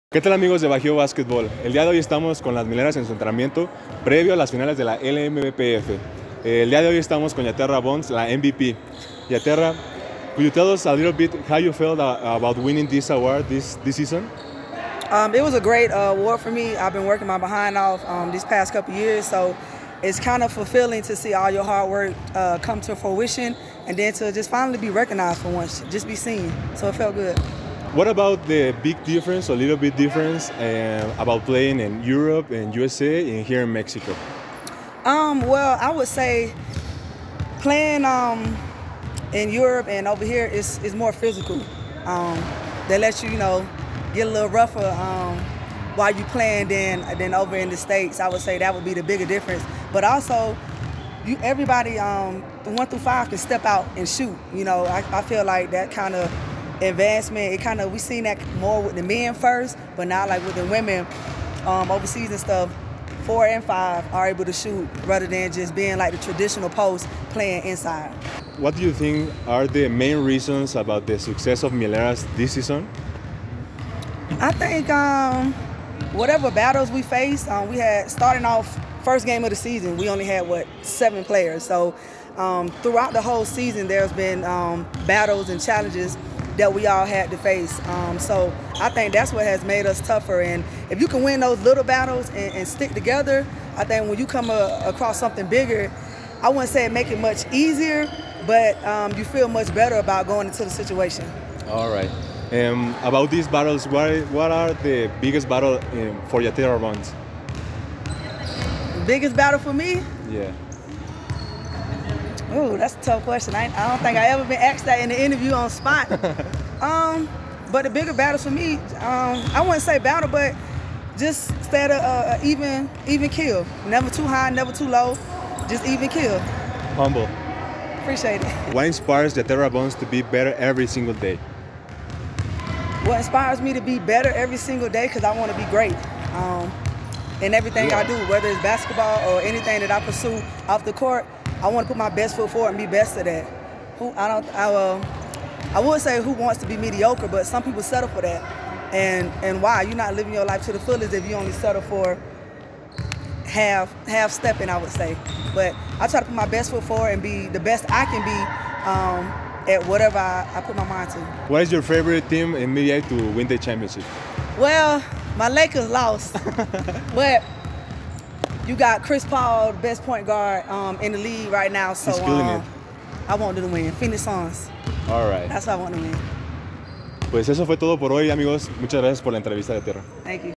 Entrevista 🔥Jaterra Bonds🔥 previo a las FINALES2021 LMBPF
"¿Cuál es la batalla más grande para mí? Mantenerme en un nivel estable, nunca muy alto nunca muy bajo."  Balance ☯️

En esta ocasión tuvimos el honor de entrevistar a la MVP de la temporada 2021 de la LMBPF: Jaterra Bonds.